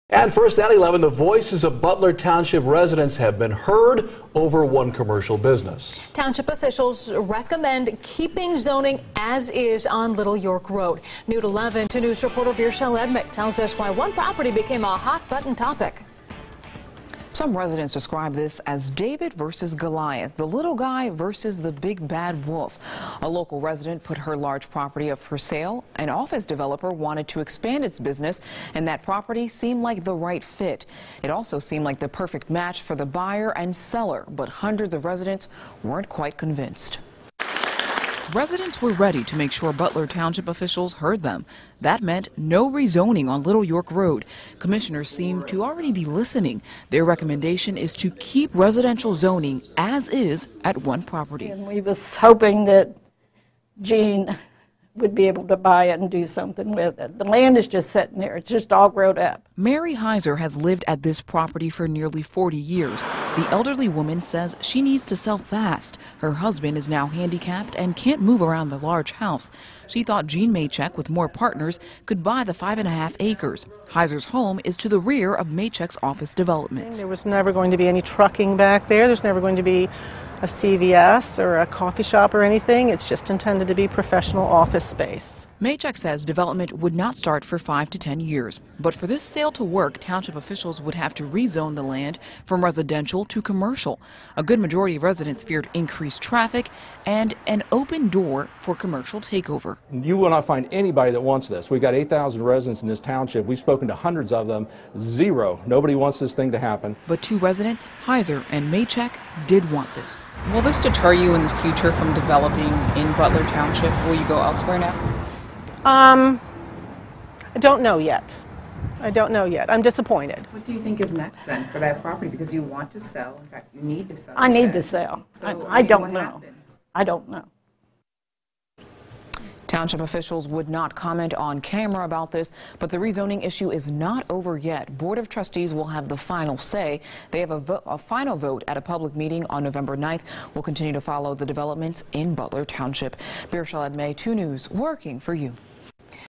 Butler Township claim victory against rezoning
Township officials recommended keeping zoning as residential on one property on the 3600 block of Little York Road.